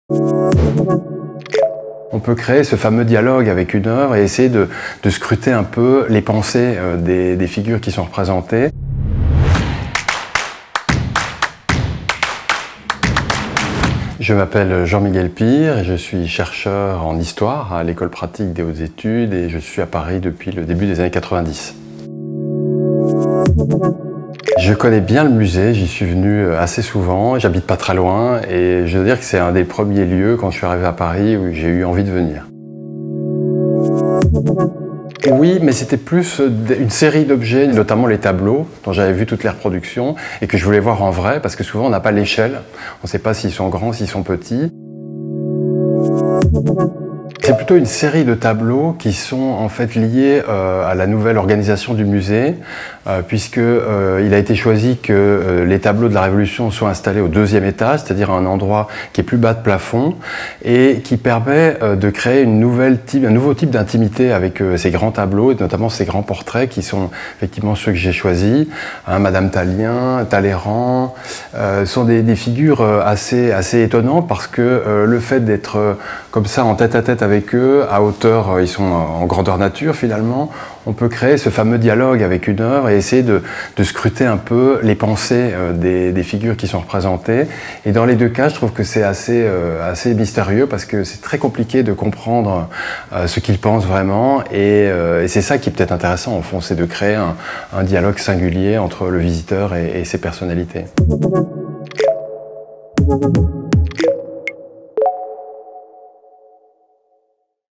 Paroles Parisiennes, S1 – Ep 7 : Jean-Miguel | Madame Tallien et Talleyrand
Dans ce septième épisode, Jean-Miguel nous propose d’entrer en dialogue avec deux œuvres du musée.

Dans de courtes vidéos, Laura, Virgile, Saadi, Charlotte, Mathusan, Féfé, Arsène, Gabrielle, Sandra et Jean Miguel partagent leur regard sur une œuvre de leur choix. Ils racontent leurs émotions, leurs souvenirs, leur lien avec Paris. Ces « paroles parisiennes » recueillies dans les salles du musée viennent ainsi donner vie aux œuvres